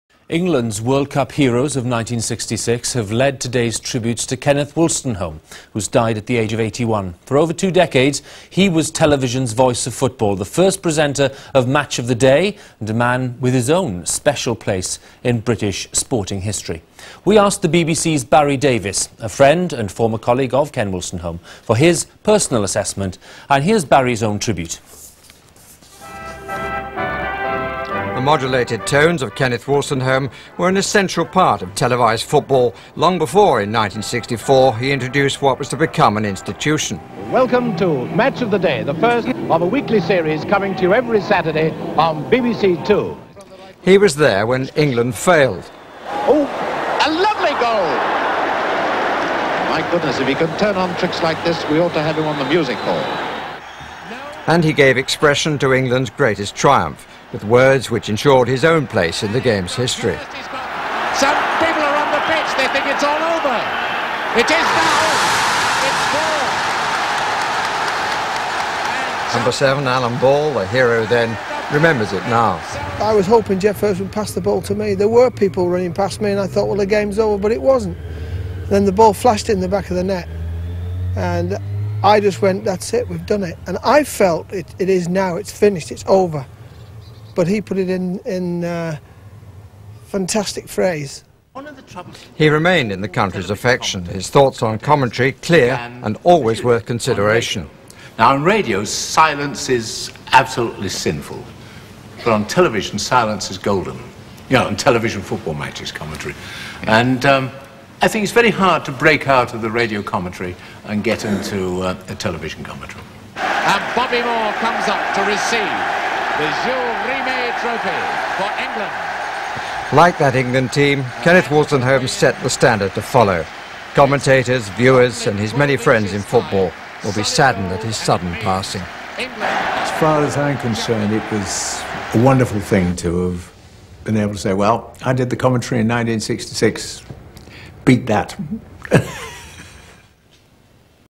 News report death of football commenter Kenneth Wolstenholme
News report from 2002 of the death of bbc football commenter